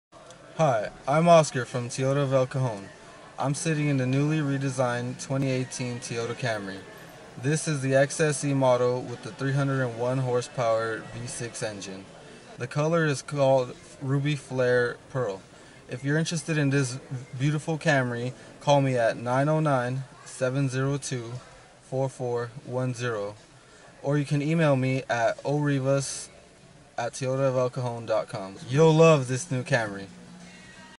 2018 Toyota Camry XSE V6
The 2018 Toyota Camry XSE V6 with 301 horsepower is one stunning sedan. The 2018 Camry is loaded with standard features and available with many options.  Our Sales Consultant Oscar Rivas has a Camry XSE to sell you.